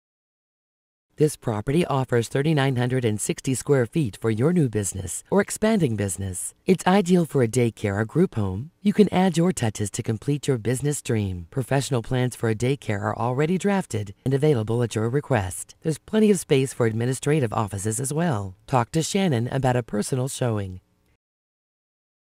Home for sale in Madisonville, OH | $170,000
3960sqft for your new or expanding business! Ideal for daycare or group home,add your touches to complete your busiess dream.Professional plans for daycare already drafted &amp; available @your request.Plenty of space for admin offices as well.